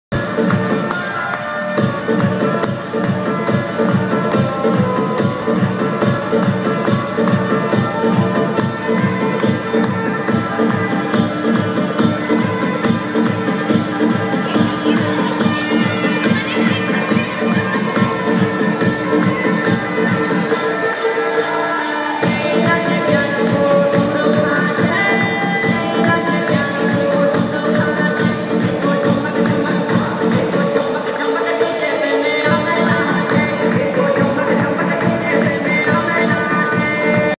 Nagori Dance
Since time immemorial, people have entertained them-self through music or dance which we call folk dance and folk music, where they have used nature to express themselves.